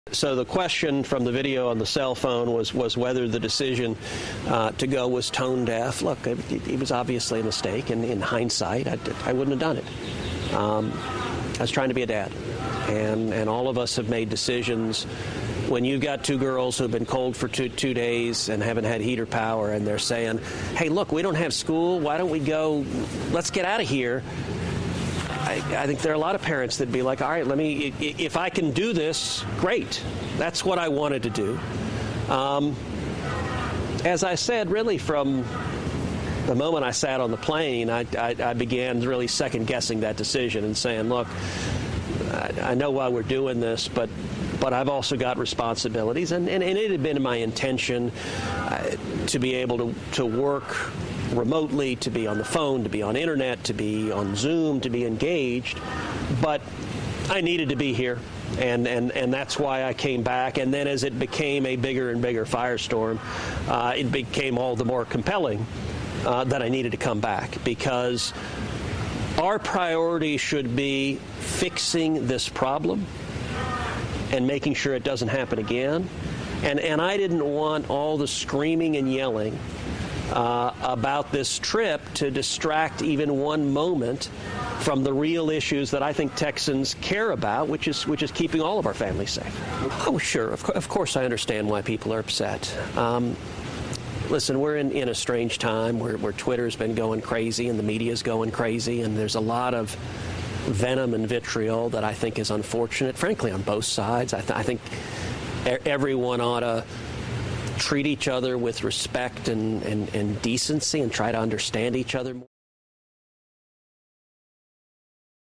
Ted Cruz claims he was being a 'good dad' for leaving Texas while millions were left without power
The Texas senator Ted Cruz has claimed he was just being “a good dad” for taking a controversial family vacation in a sunny Mexican beach resort as his home state faced a deadly winter storm that left millions without power or water.

The firebrand Republican was returning to Houston from Cancun on Thursday following furious criticism of his trip and confirmation from the city’s police chief that officers were employed to help speed his passage through the airport before his outward flight the day before.